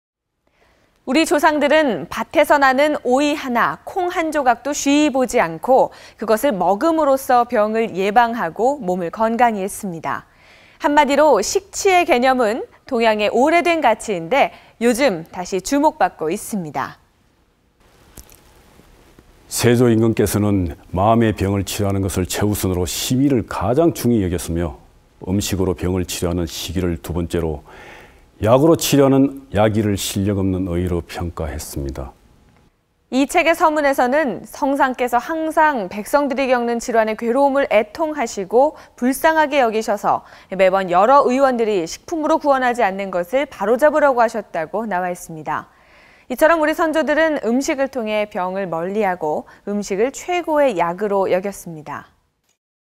음식을 최고의 약으로 여긴 선조들의 지혜 - (2011.10.8_388회 방송)_세종대왕 다이어트
[세종대왕 다이어트]

[ 원고정보 ]
우리 조상들은 밭에서 나는 오이 하나, 콩 한조각도 쉬이 보지 않고 그것을 먹음으로써 병을 예방하고 몸을 건강히 했습니다. 한마디로 식치의 개념은 동양의 오래된 가치인데 요즘 다시 주목받고 있습니다. (세조 임금께서는 마음의 병을 치료하는 것을 최우선으로 ‘심의’를 가장 중히 여겼으며 음식으로 병을 치료하는 ‘식의’를 두 번째로, 약으로 치료하는 ‘약의’를 실력 없는 어의로 평가했습니다.) 이 책의 서문에서는 성산께서 항상 백성들이 겪는 질환의 괴로움을 애통하시고 불쌍하게 여기셔서 매번 여러 의원들이 식품으로 구원하지 않는 것을 바로잡으라고 하셨다고 나와 있습니다. 이처럼 우리 선조들은 음식을 통해 병을 멀리하고 음식을 최고의 약으로 여겼습니다.